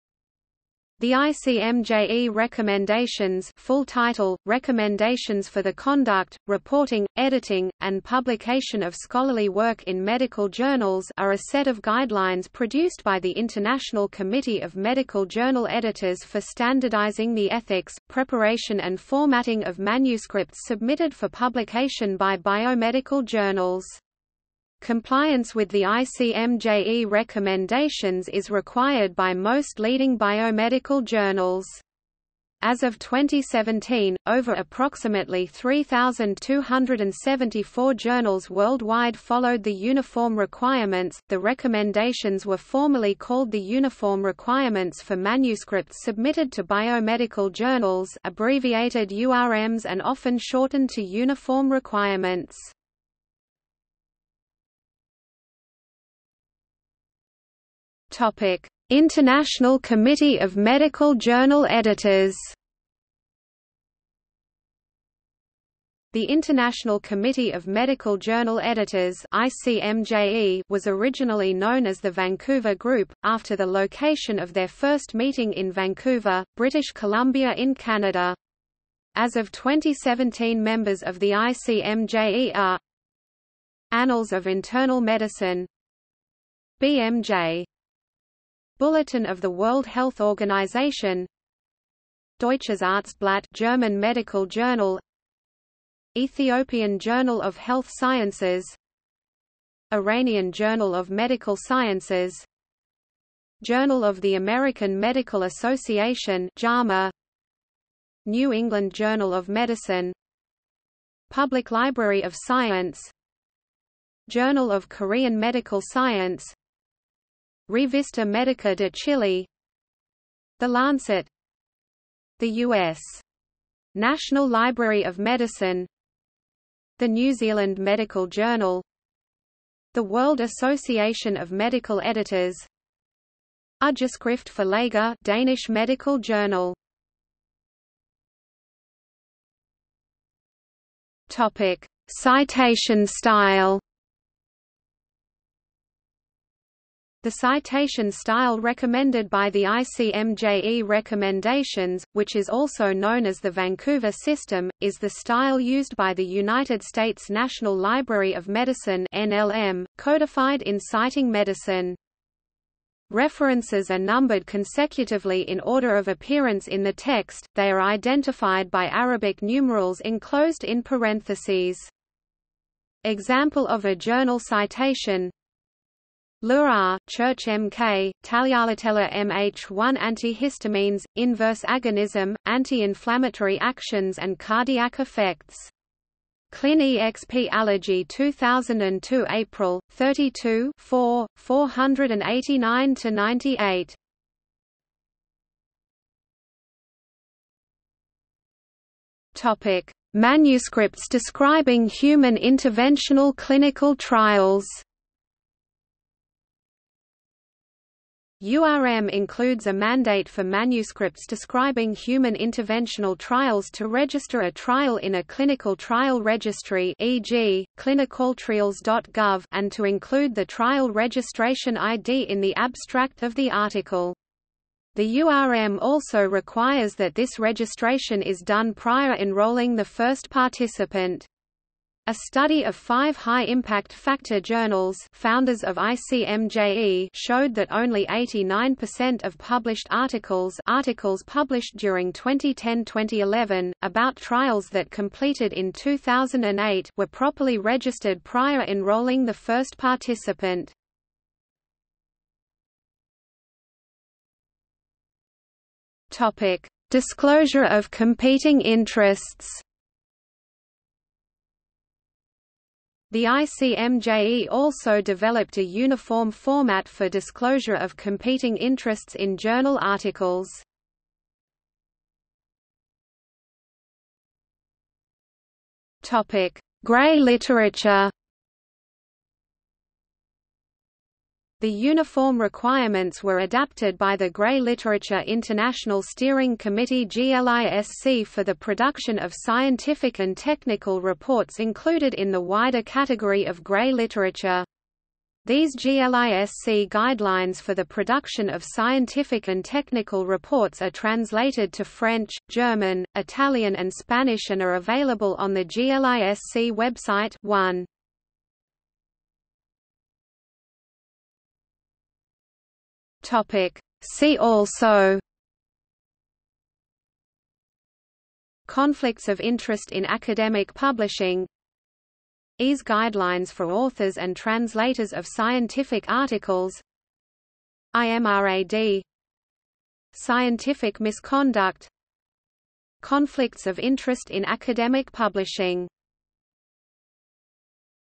Uniform Requirements for Manuscripts Submitted to Biomedical Journals | Wikipedia audio article
00:00:54 1 International Committee of Medical Journal Editors
00:02:17 2 Citation style
00:03:15 3 Manuscripts describing human interventional clinical trials
00:04:12 4 Disclosure of Competing Interests
00:04:32 5 Grey literature
00:05:09 6 See also

- Socrates

SUMMARY
The ICMJE recommendations (full title, Recommendations for the Conduct, Reporting, Editing, and Publication of Scholarly Work in Medical Journals) are a set of guidelines produced by the International Committee of Medical Journal Editors for standardising the ethics, preparation and formatting of manuscripts submitted for publication by biomedical journals. Compliance with the ICMJE Recommendations is required by most leading biomedical journals. As of 2017, over ~3274 journals worldwide followed the Uniform Requirements.The recommendations were formerly called the Uniform Requirements for Manuscripts Submitted to Biomedical Journals (abbreviated URMs and often shortened to Uniform Requirements).